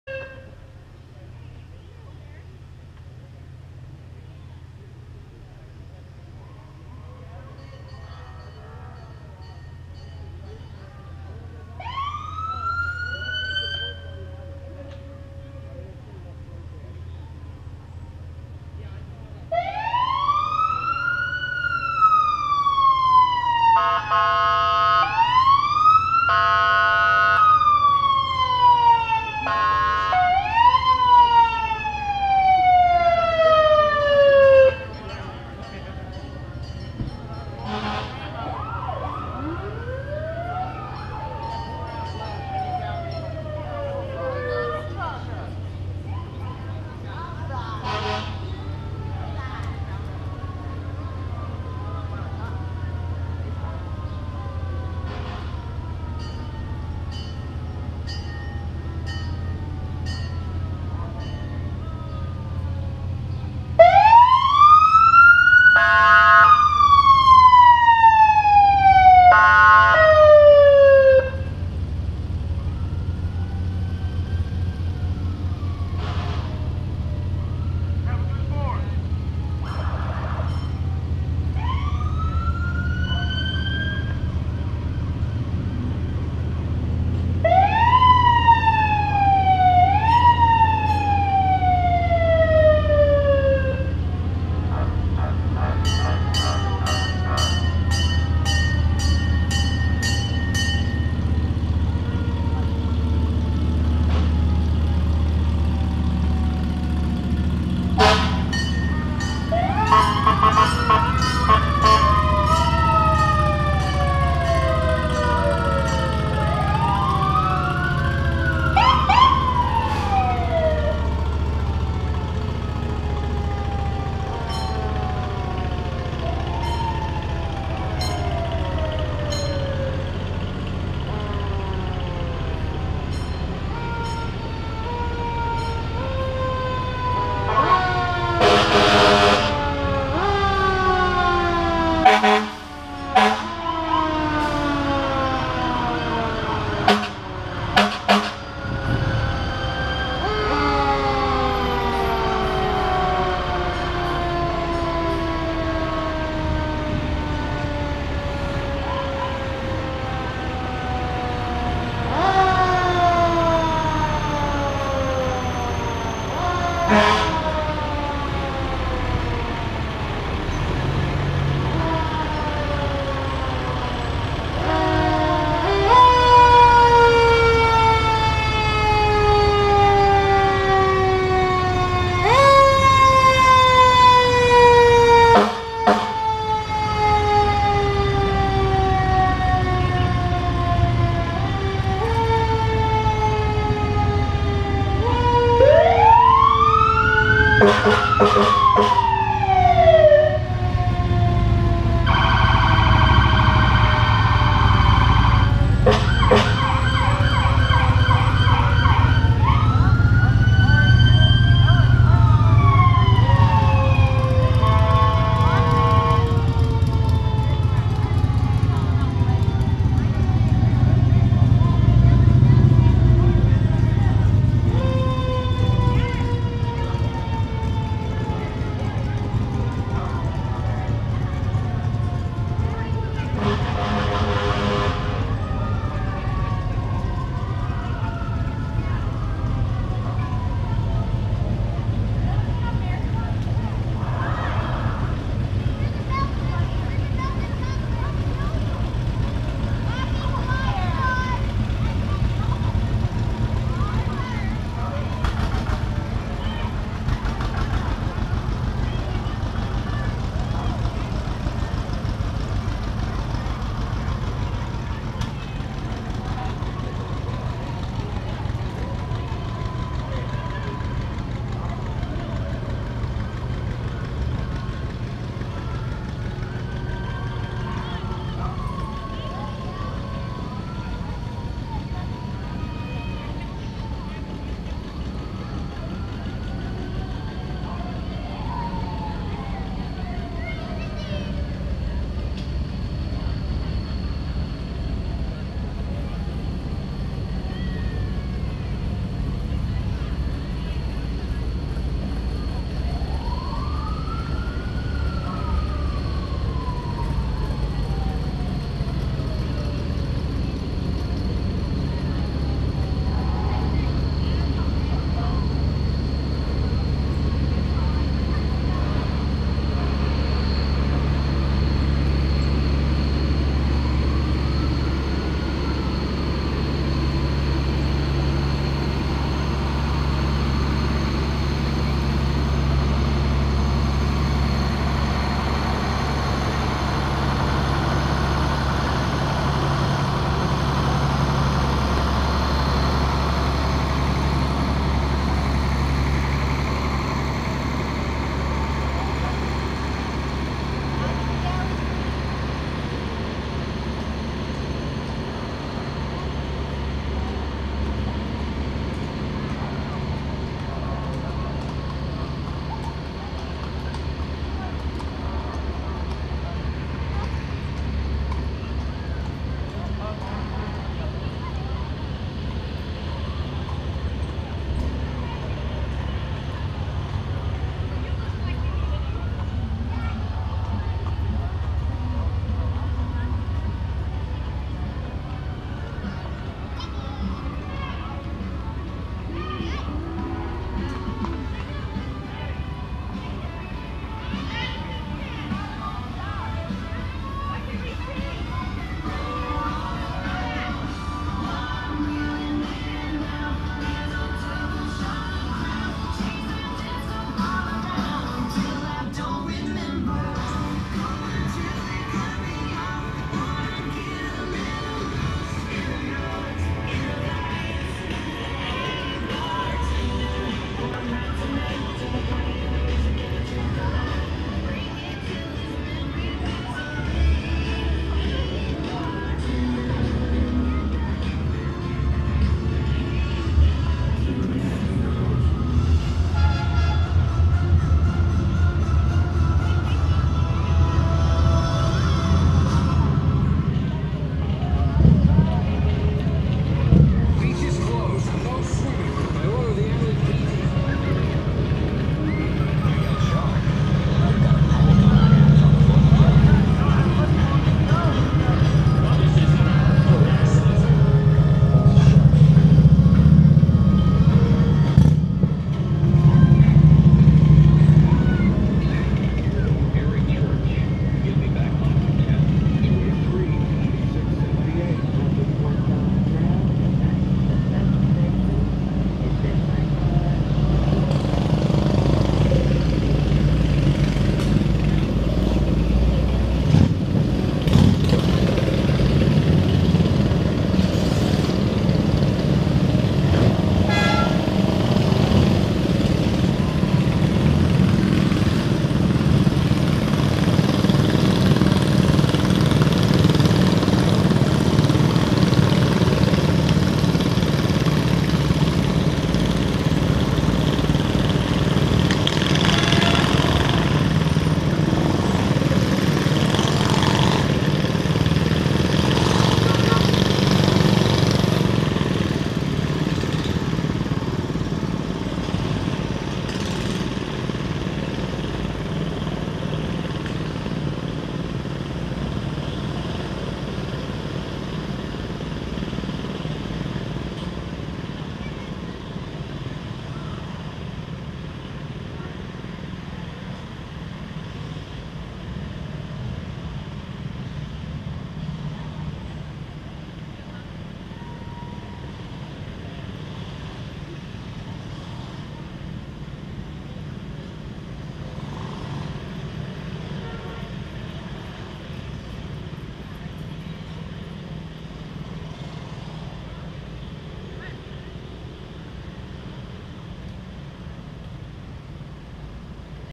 P-town Parade! 4th at the Falls
*PARADE STARTS AROUND :25 MARK* Moved recently, discovered my house was on a Parade route for the 4th of July.